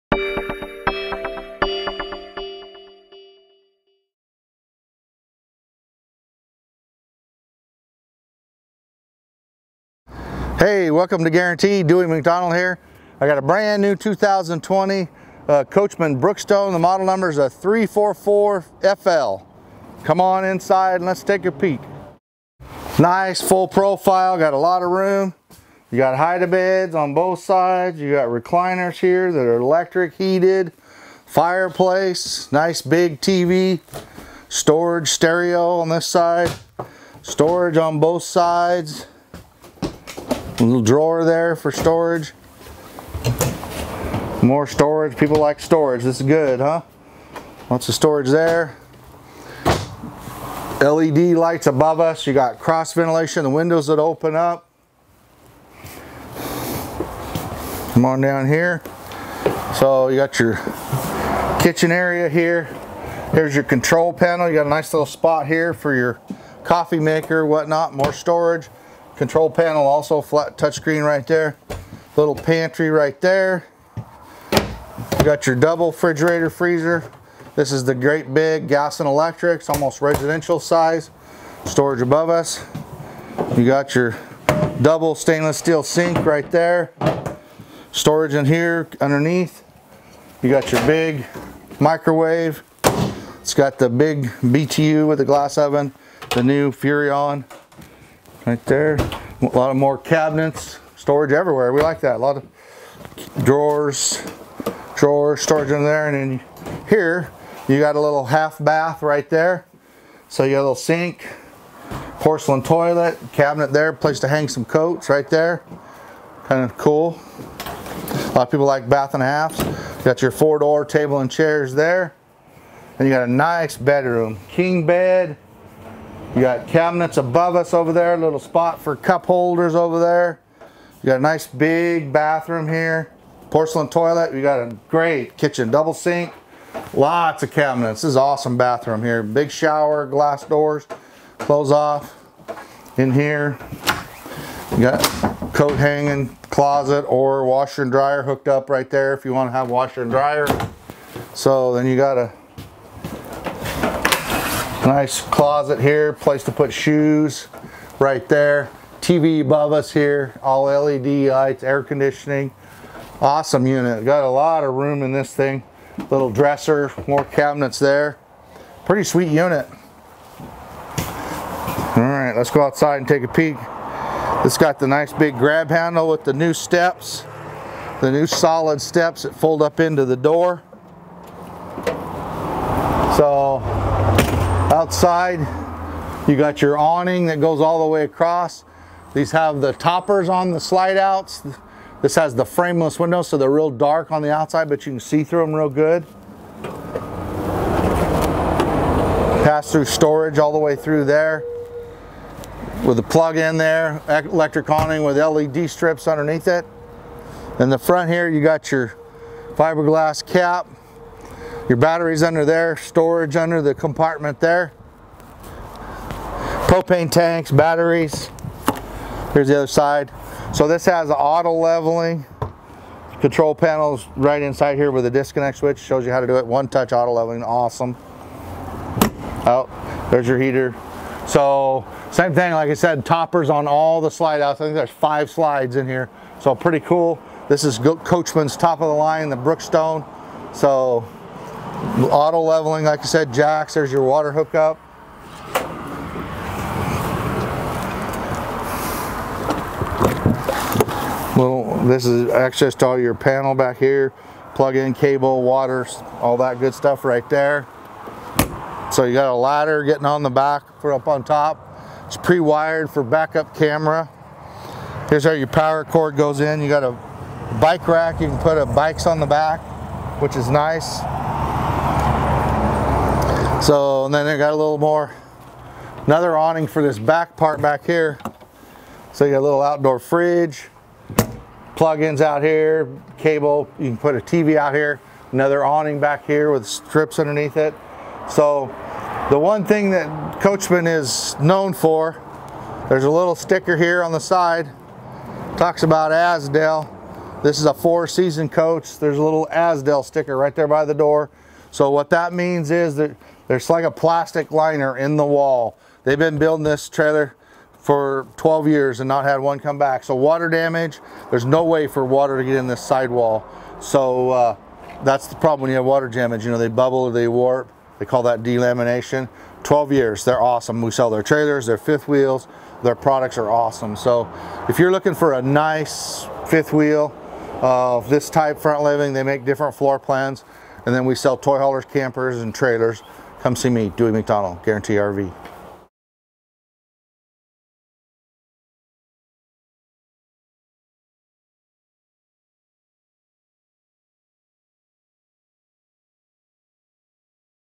2020 Coachmen Brookstone 344 DL Fifth Wheel • Guaranty.com
If you’re in the market for a Fifth Wheel, this 2020 Coachmen Brookstone 344FL could be what you’re looking for. This Brookstone Fifth Wheel peacefully sleeps 8. And know that your family will have good night sleep with: 2 sofa bed(s) and 1 queen bed. And a master bedroom conveniently located in the Center. Take your camping experience to the next level with the Brookstone Fifth Wheel which offers plenty of conveniences like a Center kitchen with Chairs, Front living area and Rear / Center bathroom. It doesn’t stop there, this Brookstone 344FL Fifth Wheel intensifies your RV camping experience, and features a stove with 3 oven burners, Full-Size refrigerator, 1 TVs, a radio Standard Fireplace and more. This Brookstone 344FL, has a GVWR of 15000 lbs and a payload capacity of 2211 lbs. And carries 1 Black Water Holding Tank(s), 1 Fresh Water Holding Tank(s), and 1 Gray Water Holding Tanks. This Brookstone has 1 door(s), 1 awning(s), 5 slideout(s). Call Guaranty RV Fifth Wheels at or visit us at 325 West 1st Avenue, Junction City, OR 97448 to schedule a showing of the Coachmen Brookstone 344FL.